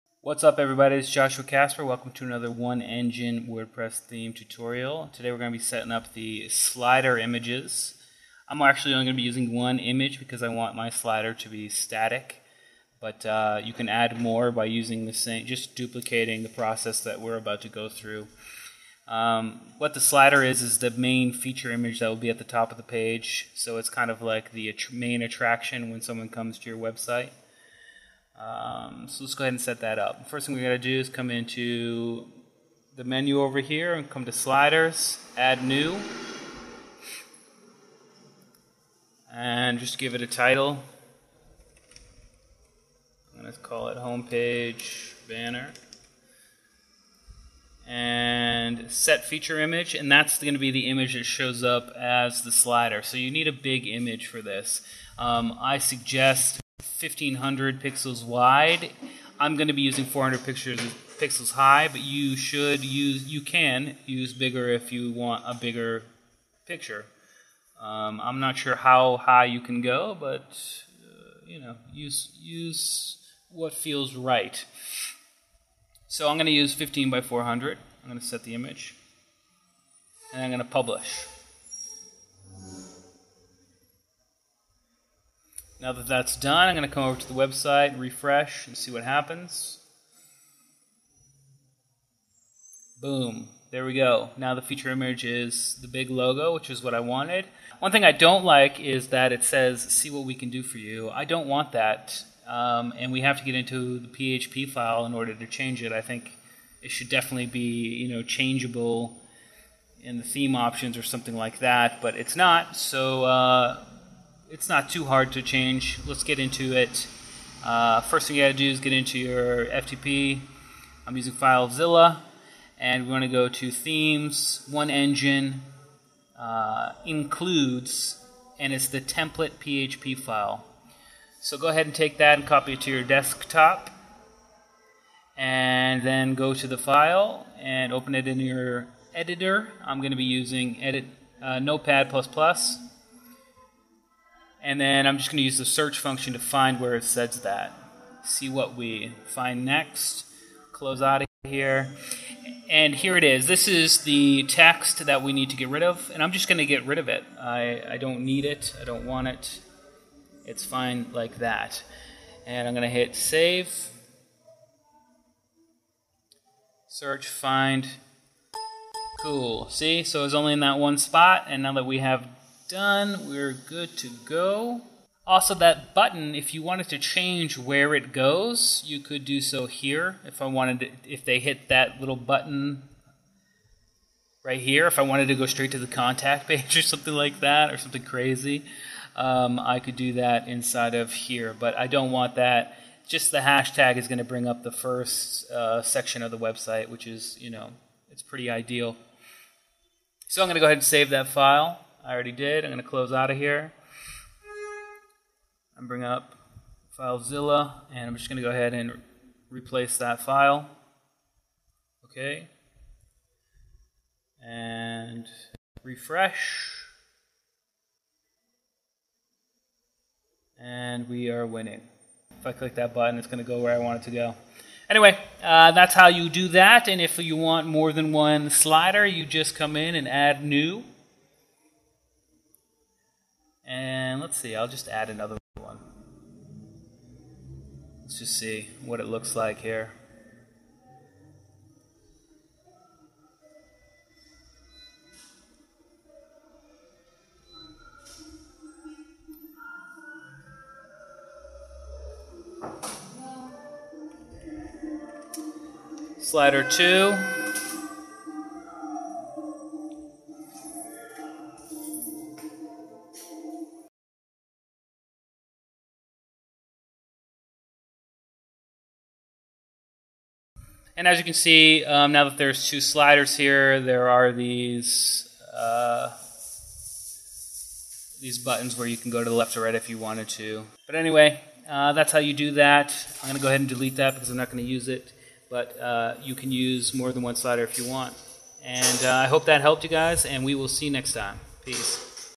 One Engine Tutorial: Sliders & Unwanted Text
One Engine video tutorial on adding feature images (sliders) to the website. Also, how to change or remove the text over the images.